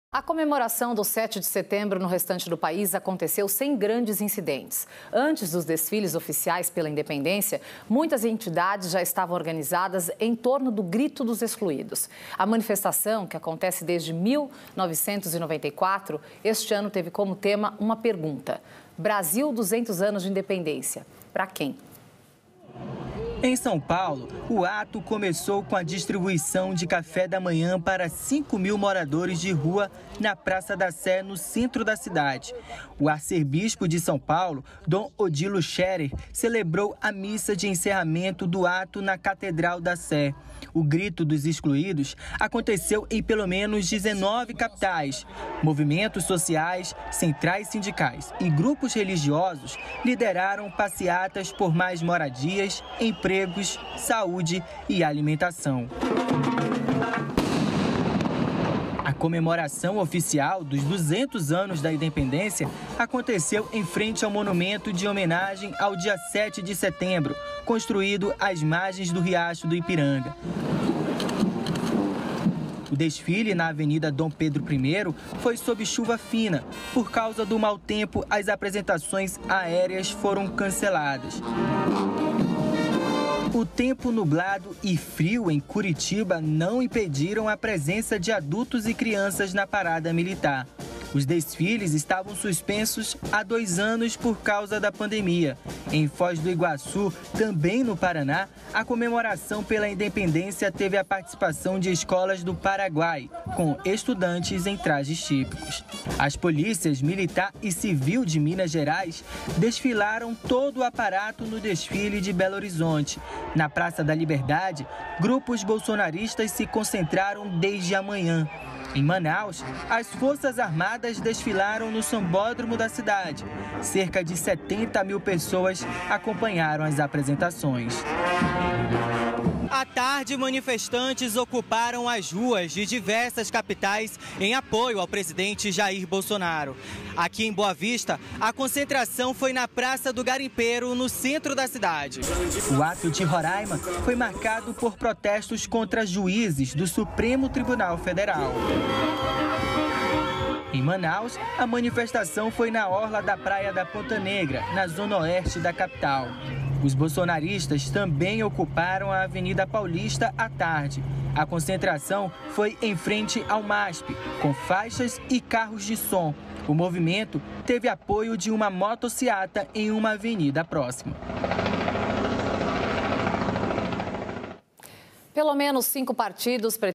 Grito dos Excluídos protesta contra fome e distribui café da manhã para moradores de rua
O 7 de setembro foi marcado por desfiles e manifestações em todo o país.

Em São Paulo, antes do evento, entidades se uniram em torno do Grito dos Excluídos, protesto contra a fome que ocorre desde 1994.